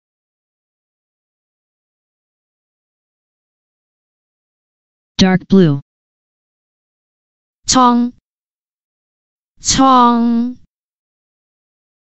"DARK BLUE" in Cantonese (蒼) - Flashcard
[chong1] "DARK BLUE" in Cantonese (蒼) - Flashcard. Additional information about the word: dark blue,deep green,housefly (Musca domestica),surname Cang.